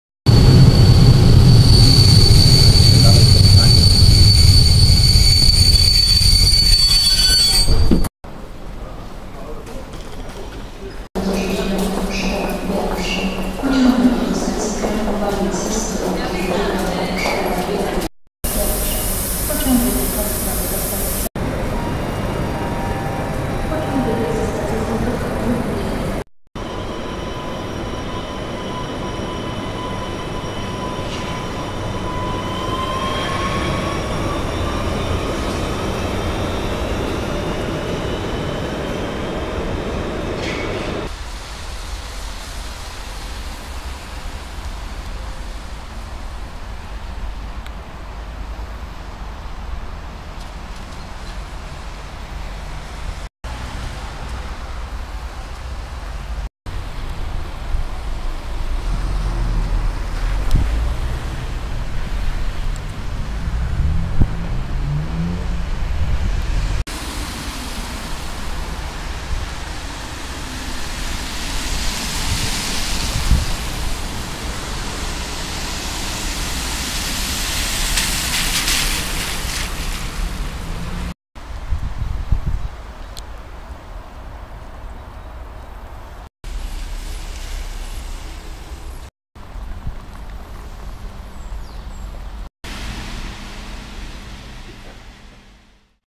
Kraków Balice Airport Train from City Centre
Cracow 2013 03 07, Poland:
Train to Kraków Balice Airport from Kraków Główny main railway station.

Kraków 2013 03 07, Polska:
Pociąg do lotniska Kraków Balice z głównego dworca kolejowego Kraków Główny.